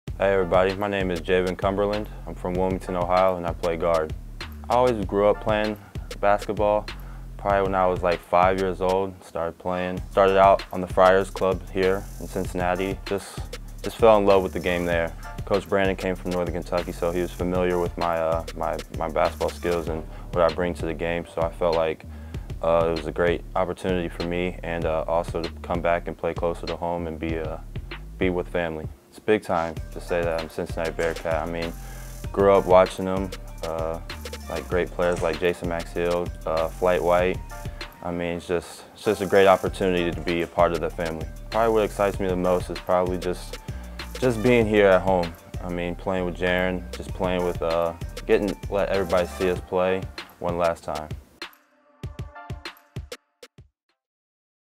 Cincinnati Men's Basketball: Meet Jaevin Cumberland
We introduce you to one of the seven newcomers on the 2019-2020 Cincinnati Men's Basketball roster, Jaevin Cumberland.